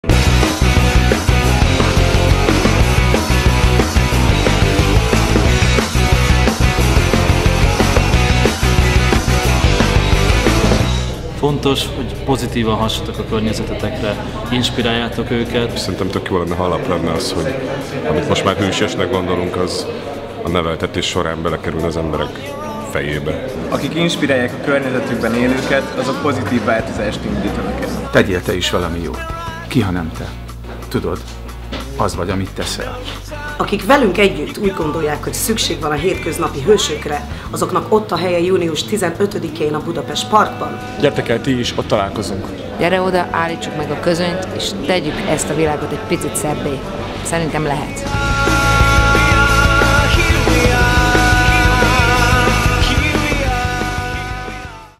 HELLÓ HŐSÖK FESZTIVÁL - 2014.06.15. Budapest PARK
Moderátorok:
Buda Márton (Petőfi Rádió)
Péter Petra (Jazzy Rádió)

Zenekarok után: Tesco Disco

Hősprogramjaink délután 4-től, nagyszínpadi műsoraink 5.30-tól kezdődnek. Gyere és bulizz velünk, DJ-ink hajnalig pörgetik a lemezeket.